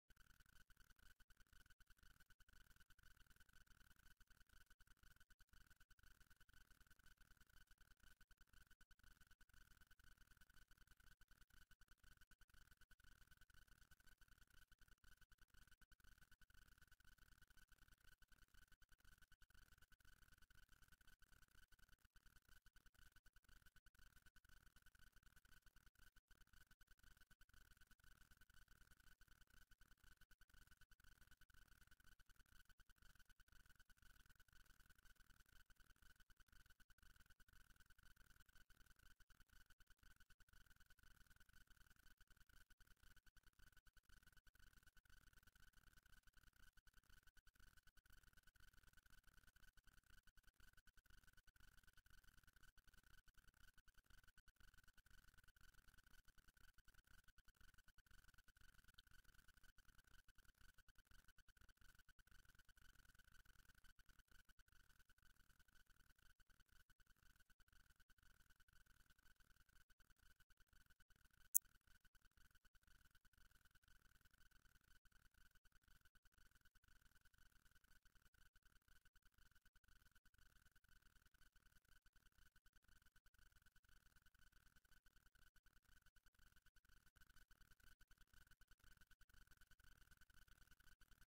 乳歯抜歯後のレーザー止血と保隙 Laser hemostasis and retention gap after extraction of milk tooth . Opelaser NEOS
子供の乳歯を抜いて、将来永久歯が生えるための場所を確保する装置を装着しています。